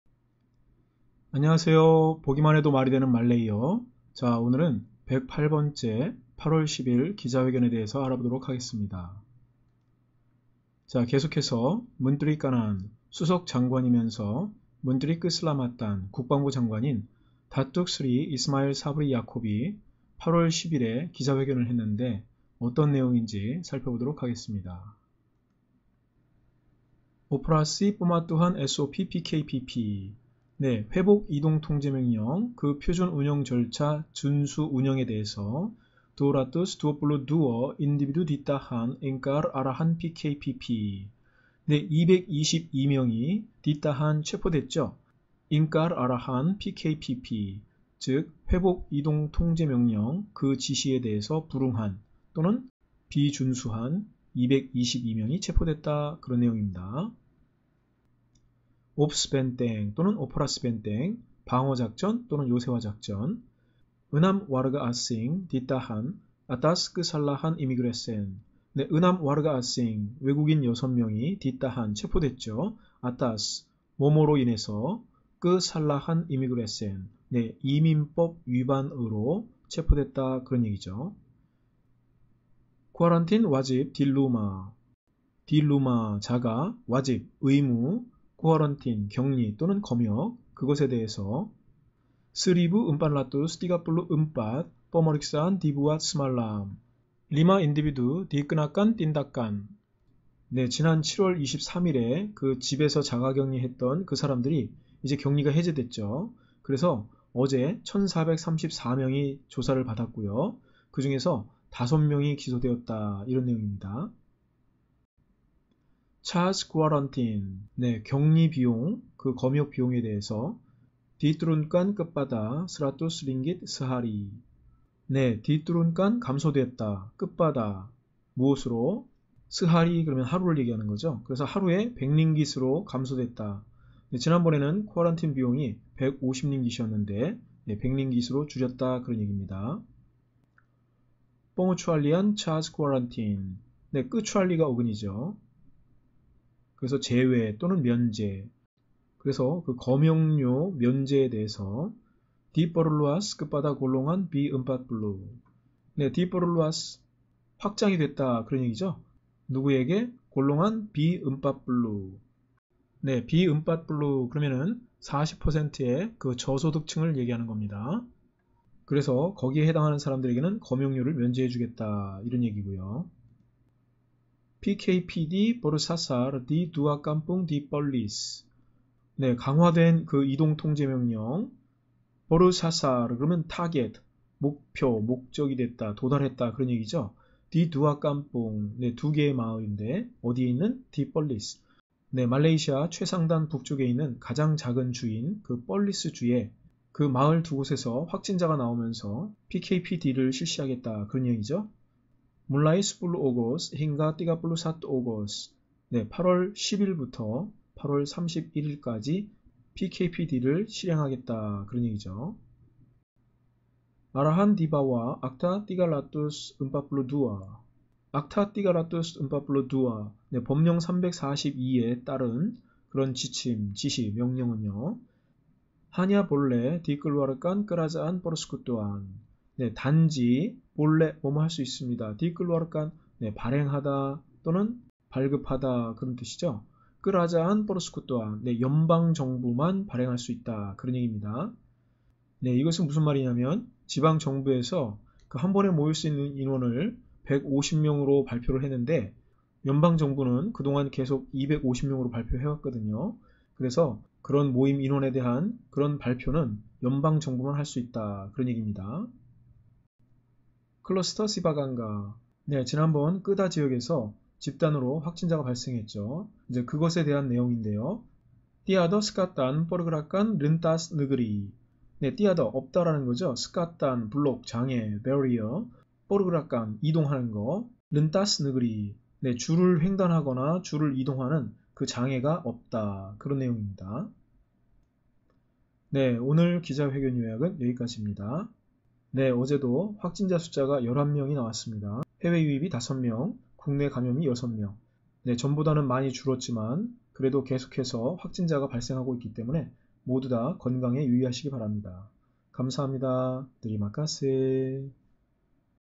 말레이시아 MCO #108,이동제한,이동통제,국가안전보장회의,말레이시아,말레이어,Malaysia mco,Malaysia language,쿠알라룸푸르,kuala lumpur
코로나로 인해 'PKPP(회복이동통제명령)'이 실시되고 있습니다. 오늘은 8월 10일에 공지한 내용을 알아보겠습니다.^^